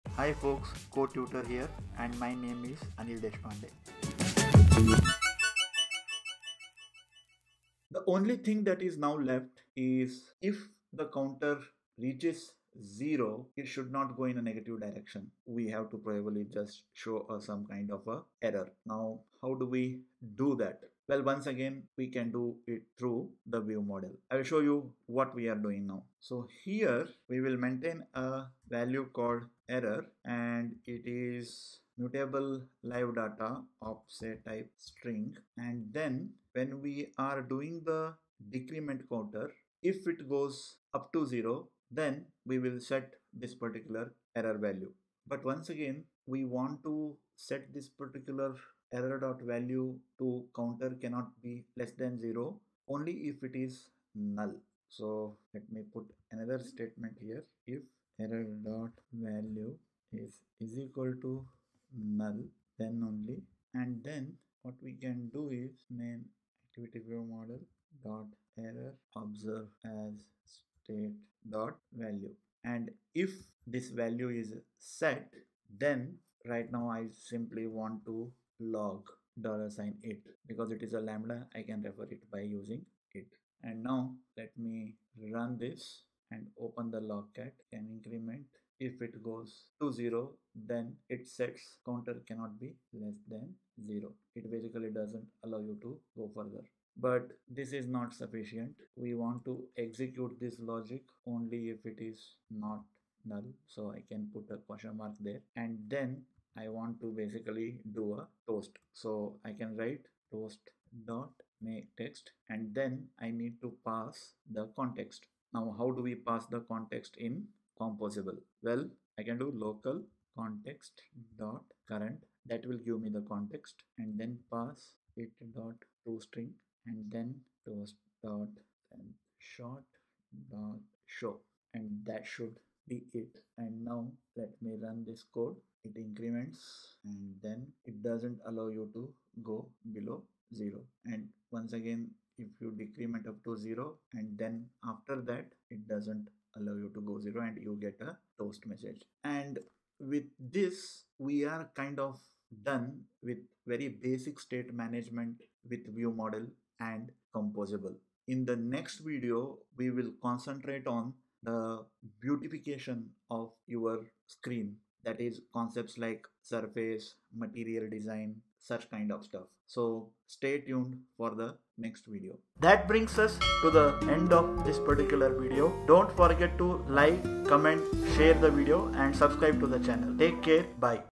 Jetpack Compose State Management | Fixing UI Errors with ViewModel! | Jetpack Compose 12
In this video, we explore state management in Jetpack Compose using ViewModel. We implement a simple logic to prevent the counter from going negative and display an error message when the decrement button is clicked at zero.

What You’ll Learn:

✅ How to handle state management in Jetpack Compose
✅ Using ViewModel and MutableLiveData for UI state updates
✅ Observing state changes with observeAsState()
✅ Showing Toast messages in a composable function
✅ Using LocalContext.current to access context in Jetpack Compose

By the end of this video, you’ll have a solid understanding of handling UI states and preventing invalid interactions in Jetpack Compose.